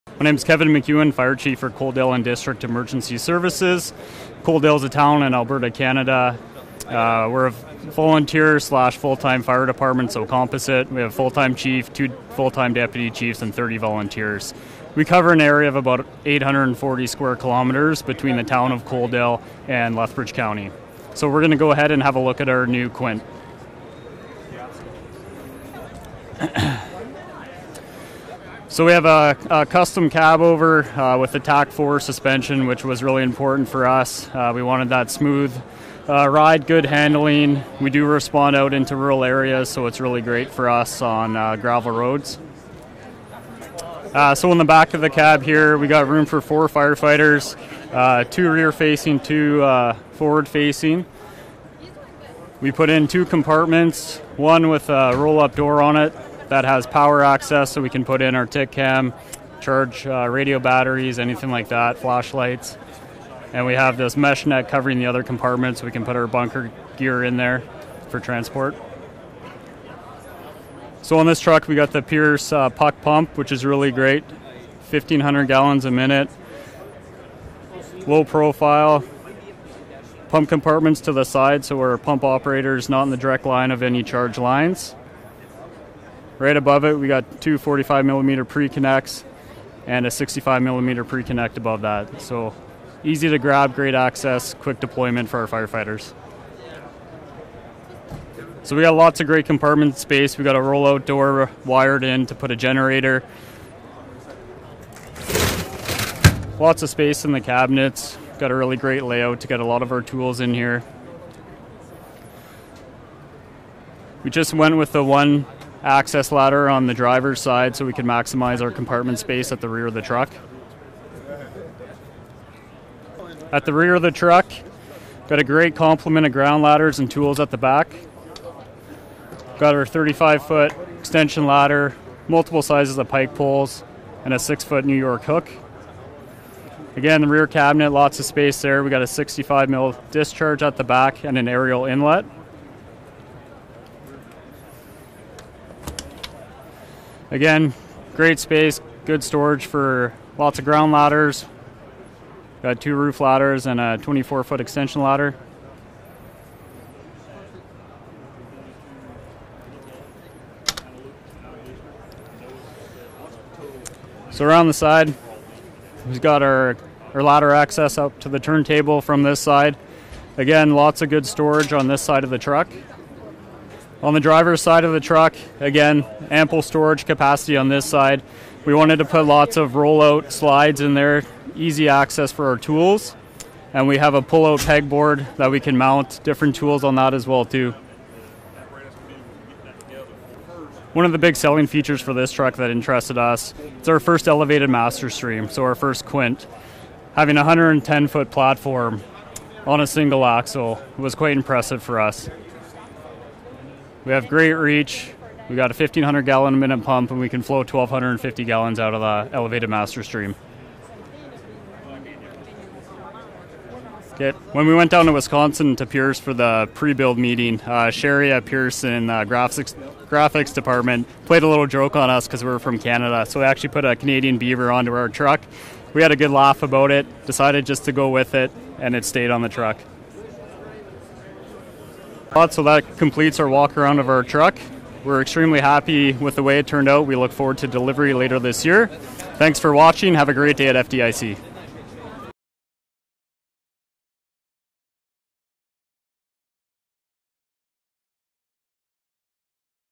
Enforcer™ Ascendant® 110’ Single Axle Aerial Platform – Coaldale, AB
The Coaldale & District Emergency Services will be taking delivery of their very first quint. This aerial features the PUC pump. The pump compartment is to the side so that the pump operator is not in the direct line of any charge lines. The department chose to have only one access ladder to the turntable on the driver’s side, maximizing compartment space at the rear of the truck.

CHASSIS
Chassis: 70” Enforcer™ cab
Seating capacity: 6
Overall height: 12’ 3”
Overall length: 42’ 8.5”
GVW rating: 56,300 lb
Front axle: TAK-4® Independent Front Suspension, 22,800 lb
Rear axle: Meritor RS30-185, 33,500 lb
Engine: Cummins L9, 450 hp, 1,250 torque
Electrical system: Command Zone™ advanced electronics system

BODY
Material Aluminum
Shelving Adjustable, up to 500 lb
Doors Gortite roll-up
Pump: PUC™, 1,500 gpm
Tank: 500 gallons
Pump panel: Control Zone™, 36” pump panel
Foam system: Husky™ 3
Foam cell: 30 gallons

AERIAL DEVICE
Device: Ascendant 110’ platform, 4-section steel aerial
Reach: 110’ vertical, 90’ horizontal
Tip load: 750 lb dry, 500 lb wet
Equipment: 100 lb allowance
Flow rate: 1,250 gpm
Stabilizers: 1 set, 18’ spread, 1 down-jack
Platform: 3-person

Job No: 32750
Dealership: Commercial Emergency Equipment